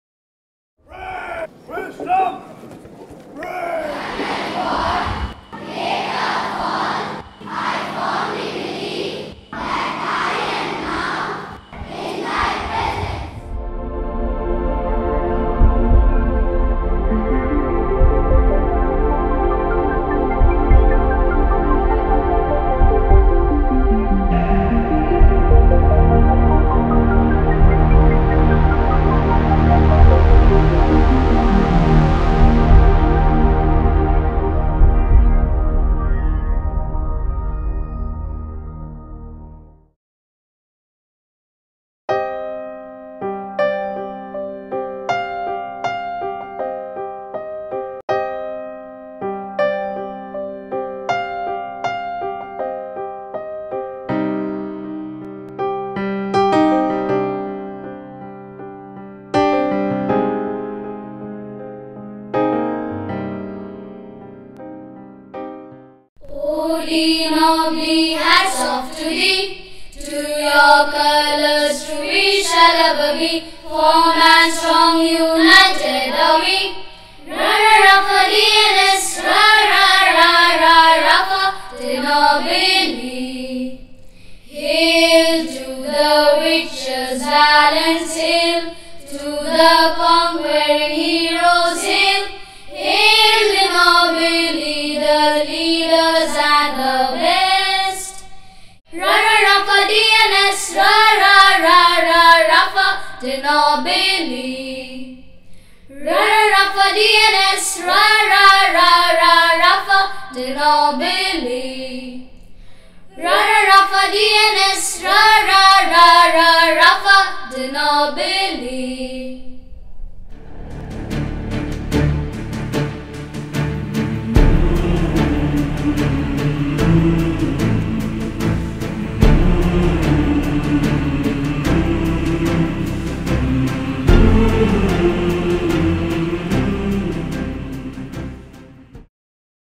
TEASER 2 NEW
A documentary in the making on Fr. Hess & De-Nobili school. Produced by batch 81-A.